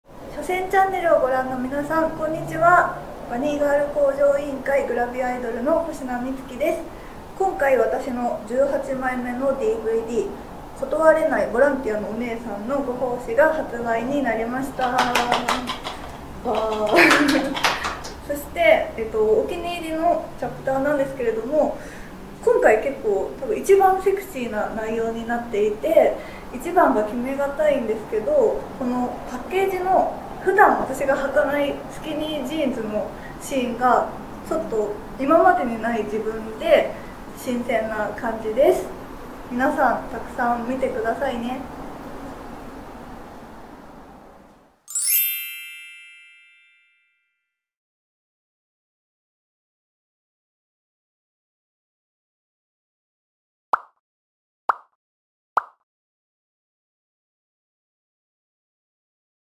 “バニーの天使” 星那美月さん 最新イメージDVD見どころ紹介！☆書泉チャンネル HD 1080p
バニーガール向上委員会 委員長、グラビアアイドルの星那美月さんをお迎えして、DVD『ことわれないボランティアのお姉さんのご奉仕』（イーネット・フロンティア）発売記念イベントを2022年3月27日に書泉ブックタワーで開催。書泉チャンネルにコメントをいただきました。

☆好評発売中
星那美月／ことわれないボランティアのお姉さんのご奉仕
価格：4,400円税込　発売日：2022年3月18日　品番：ENFD-4371発売元：ブレイン販売元：イーネット・フロンティア

◆書泉ホームページ（アイドルジャンルのフェア、イベント情報）